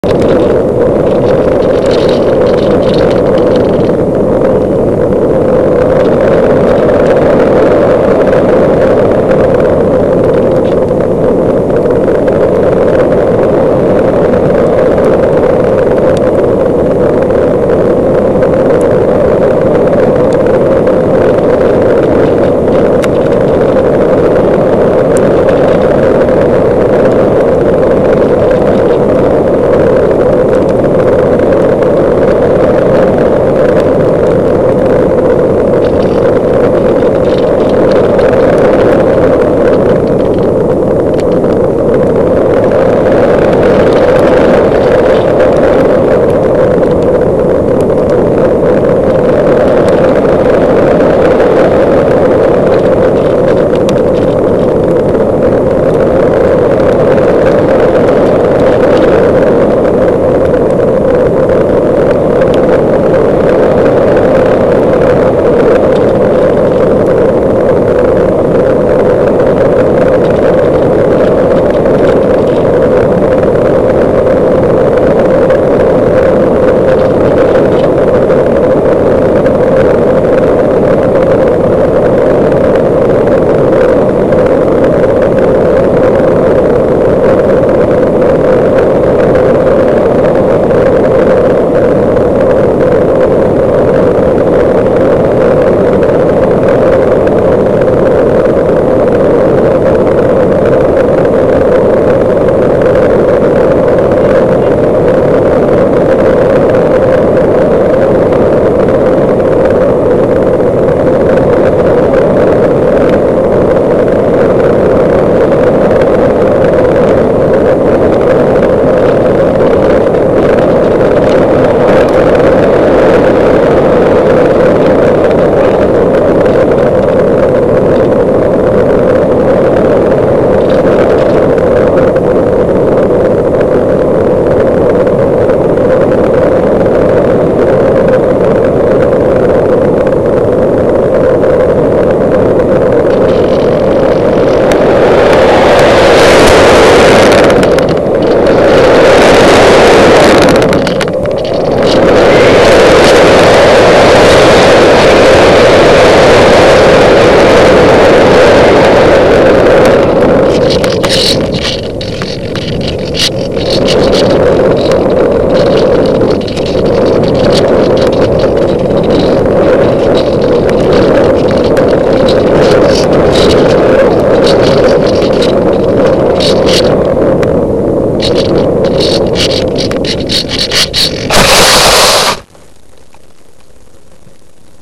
Parkzone Radian Flight over Gosport Hampshire
Just fitted a fly cam one to my Parkzone Radian so when for a flight to try it out.  So now the plan is to try and film flights in spectacular locations! We shall see.